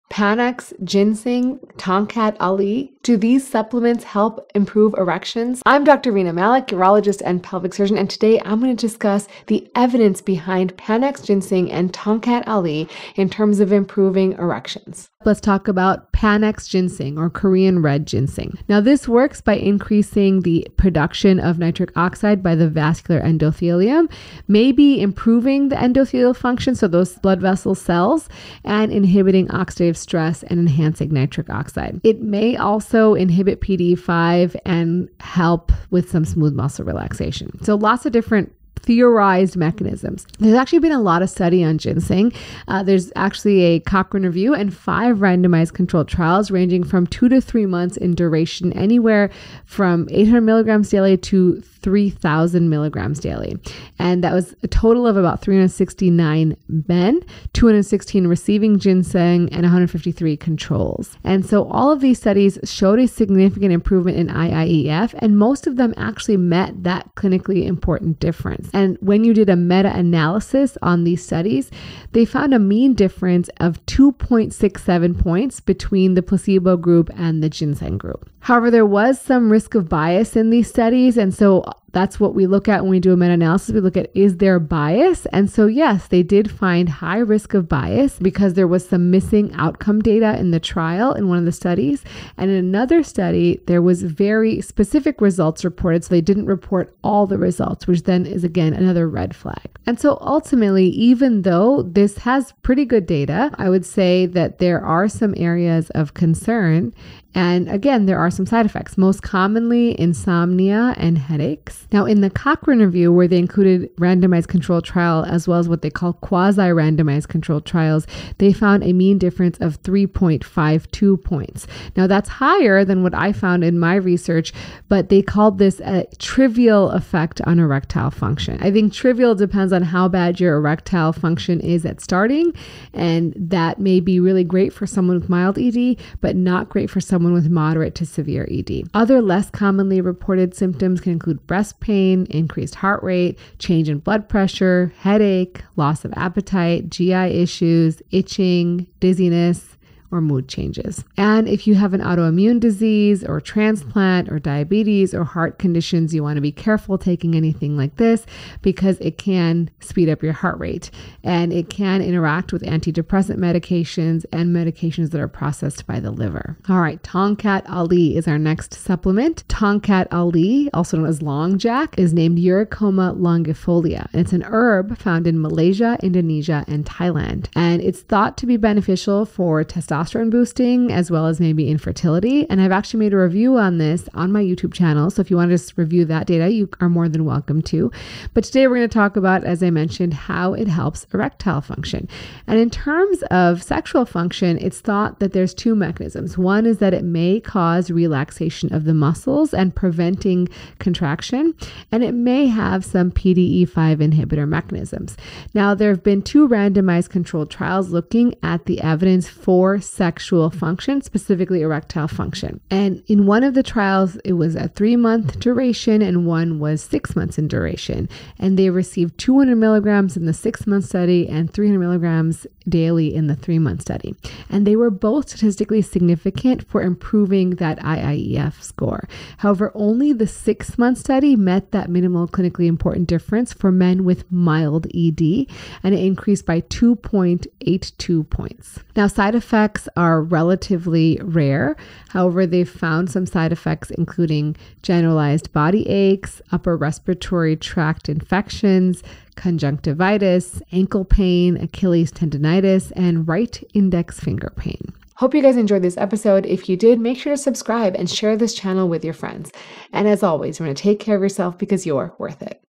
Is Panax Ginseng the Miracle Sex Pill You Want?! | Urologist Explains
In this episode, Dr. Rena Malik, MD, explores the potential benefits of Panax ginseng and Tongkat Ali for improving erectile function. She delves into the scientific evidence, including randomized controlled trials and meta-analyses, to assess their effectiveness in treating erectile dysfunction (ED). Dr. Malik discusses the various mechanisms through which these supplements may work, such as increasing nitric oxide production and inhibiting oxidative stress for Panax ginseng, and muscle relaxation and PDE5 inhibition for Tongkat Ali. She also highlights the potential side effects and precautions for individuals with specific health conditions. This episode provides a comprehensive overview for listeners interested in alternative treatments for ED and the importance of understanding the scientific evidence behind these supplements.

Download my top 10 guide to maximizing pleasure - Better Sex,Better Life

▶️Chapters:

00:00 Panax Ginseng
01:47 Side Effects
02:47 Tongkat Ali
04:03 Side Effects